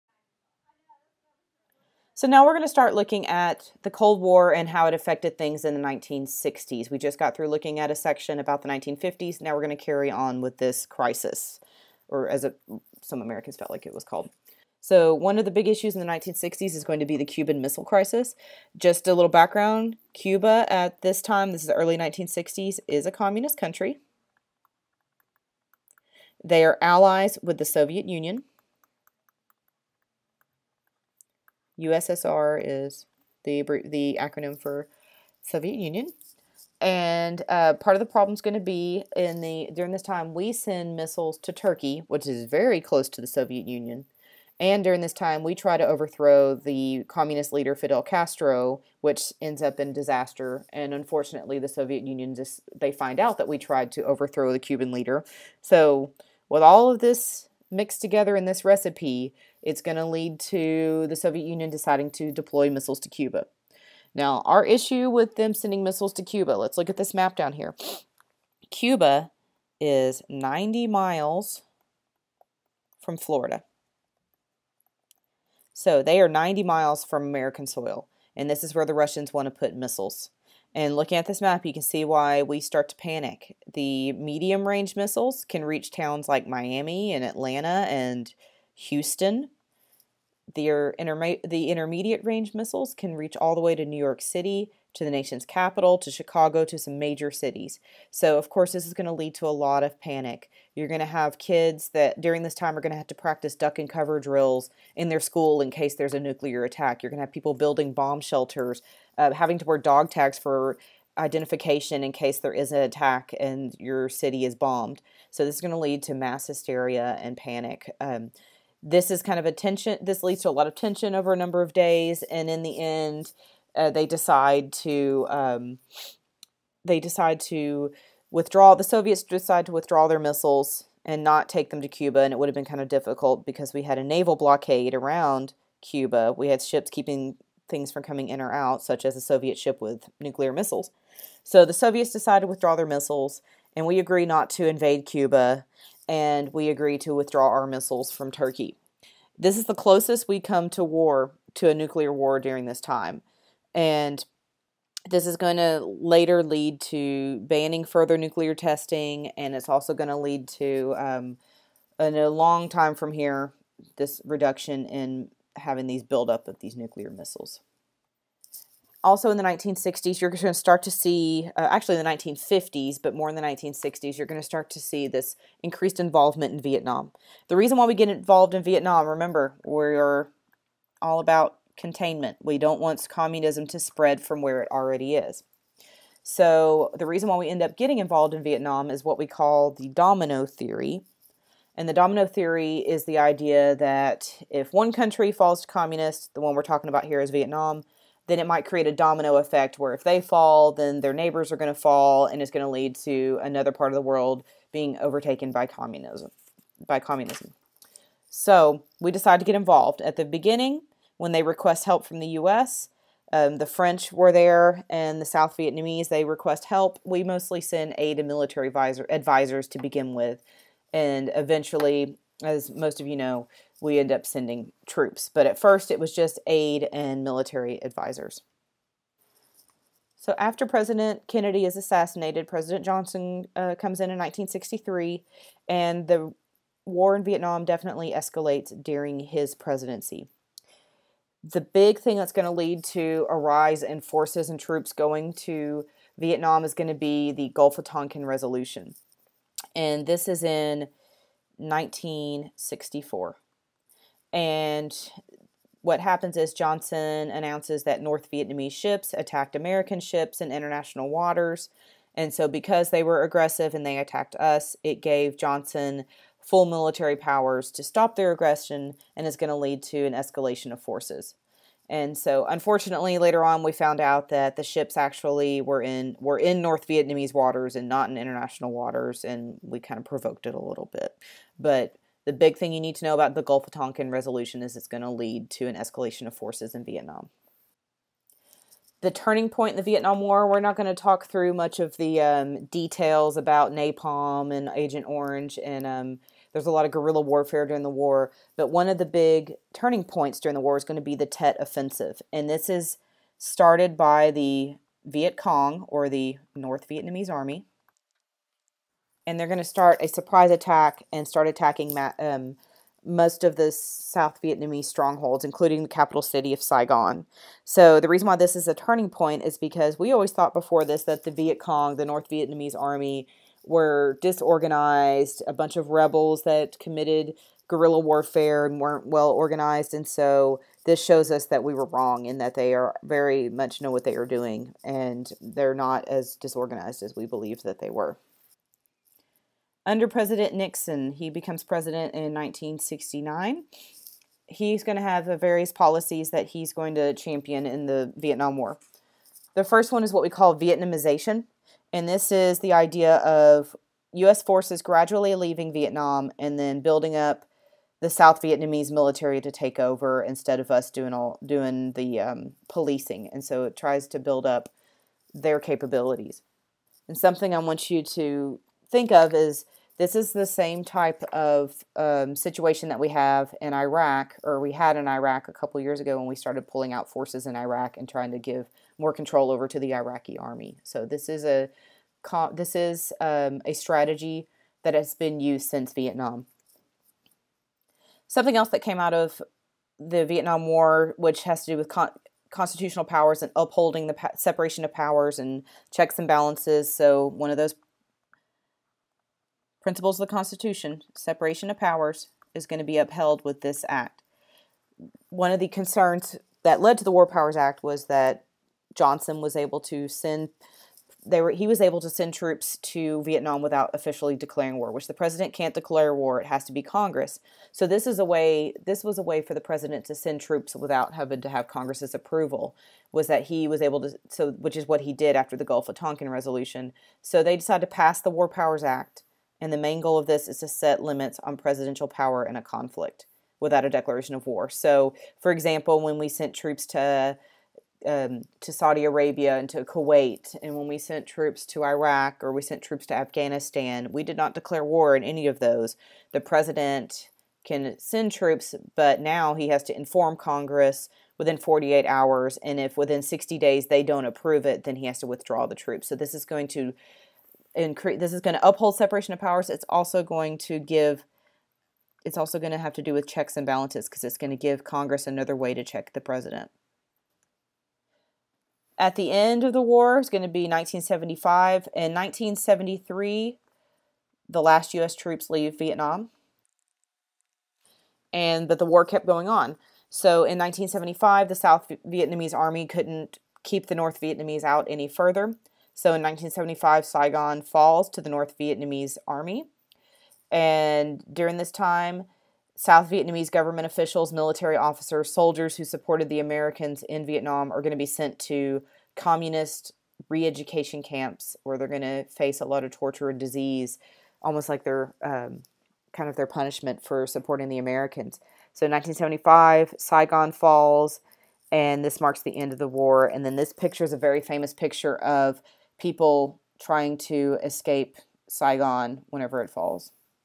Cold War EOCReviewPart2
The Cold War in the 1960's including the Vietnam War

All material is aligned to the TEKS in preparation for the Texas End-of-Course exam that all high students must pass to graduate high school.